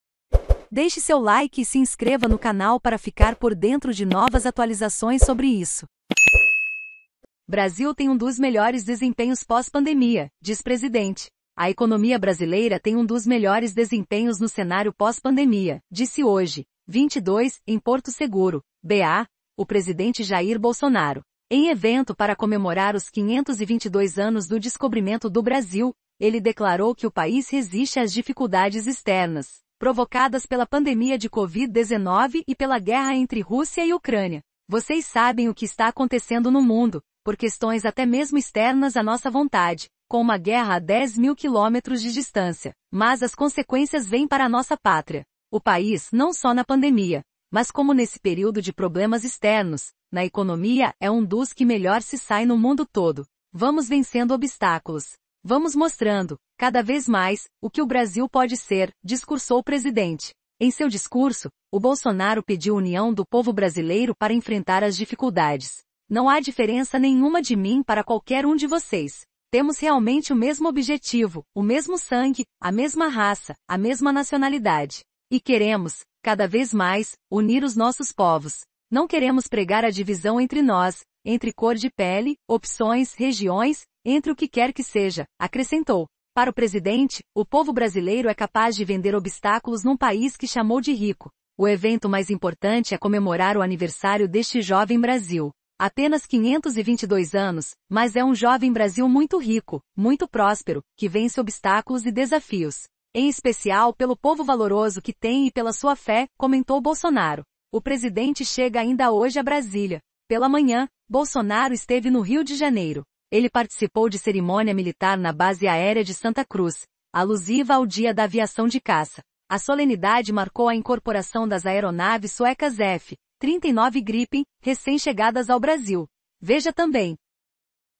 BRASIL TEM UM DOS MELHORES DESEMPENHOS PÓS PANDEMIA. DIZ PRESIDENTE
A economia brasileira tem um dos melhores desempenhos no cenário pós pandemia. disse hoje (22). em Porto Seguro (BA). o presidente Jair Bolsonaro. Em evento para comemorar os 522 anos do descobrimento do Brasil. ele declarou que o país resiste às dificuldades externas. provocadas pela pandemia de covid 19 e pela guerra entre Rússia e Ucrânia. “Vocês sabem o que está acontecendo no mundo. por questões até mesmo externas à nossa vontade. com uma guerra a 10 mil quilômetros de distância. mas as consequências vêm para a nossa pátria. O país. não só na pandemia. mas como nesse período de problemas externos. na economia é um dos que melhor se sai no mundo todo. Vamos vencendo obstáculos. Vamos mostrando. cada vez mais. o que o Brasil pode ser”. discursou o presidente. Em seu discurso. o Bolsonaro pediu união do povo brasileiro para enfrentar as dificuldades. “Não há diferença nenhuma de mim para qualquer um de vocês. Temos realmente o mesmo objetivo. o mesmo sangue. a mesma raça. a mesma nacionalidade. E queremos. cada vez mais. unir os nossos povos. Não queremos pregar a divisão entre nós. entre cor de pele. opções. regiões. entre o que quer que seja”. acrescentou. Para o presidente. o povo brasileiro é capaz de vender obstáculos num país que chamou de rico. “O evento mais importante é comemorar o aniversário deste jovem Brasil. Apenas 522 anos. mas é um jovem Brasil muito rico. muito próspero. que vence obstáculos e desafios. Em especial. pelo povo valoroso que tem e pela sua fé”. comentou Bolsonaro. O presidente chega ainda hoje a Brasília. Pela manhã. Bolsonaro esteve no Rio de Janeiro. Ele participou de cerimônia militar na Base Aérea de Santa Cruz. alusiva ao Dia da Aviação de Caça. A solenidade marcou a incorporação das aeronaves suecas F 39 Gripen. recém chegadas ao Brasil. Veja também